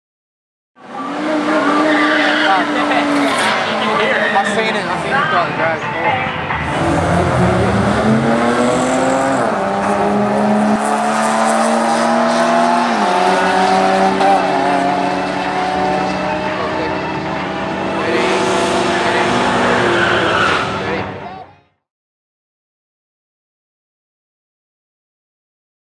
13 second Au NA 6cyl Drag run - Willowbank Test and Tune
Au Falcon showing how it is done NA 6cyl style.

Willowbank test and Tune